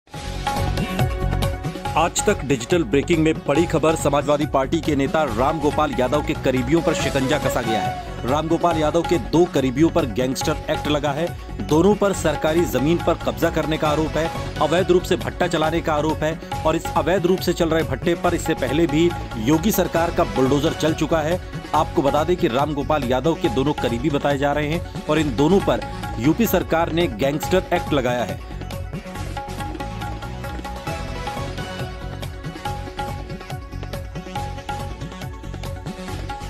SP नेता Ram Gopal Yadav के करीबियों पर लगा गैंगस्टर एक्ट! Yogi सरकार ने अवैध कब्जे पर चलवाया बुलडोज़र
सपा नेता रामगोपाल यादव के करीबियों पर कसा शिकंजा. रामगोपाल यादव के दो करीबियों पर लगा गैंगस्टर एक्ट. दोनों पर सरकारी जमीन पर कब्जा करने का आरोप. अवैध रूप से भट्ठा चलाने का है आरोप. अवैध रूप से चल रहे भट्ठे पर चल चुका है बुलडोजर.